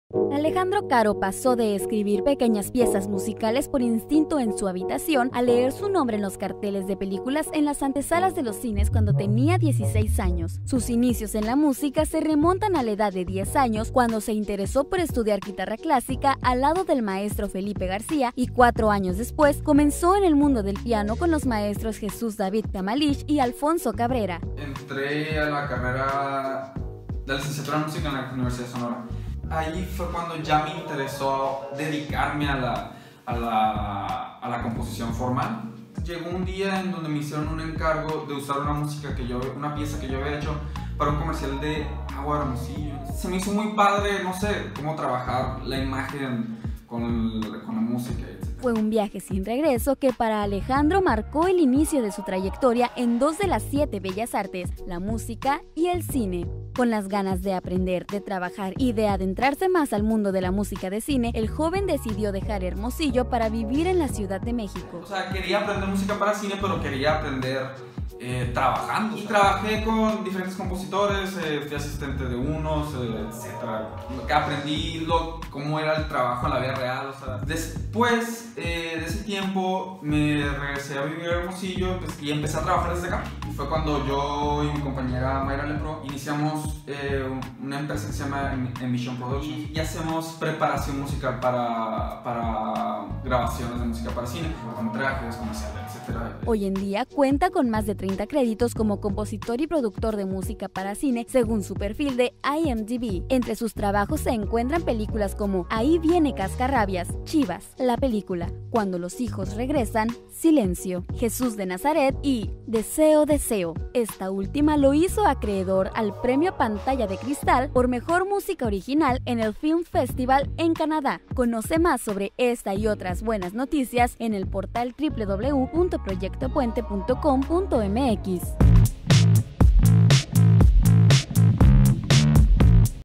El hermosillense Alejandro Karo produce soundtracks para películas
Alejandro Karo pasó de escribir pequeñas piezas musicales por instinto en su habitación, cuando tenía 16 años, a leer su nombre en los carteles de películas en las antesalas de los cines.
El joven hermosillense de 26 años se ha abierto las puertas al mundo de la composición musical cinematográfica y a su corta edad ha trabajado en cinco películas como compositor y en 11 producciones como preparador musical y orquestador.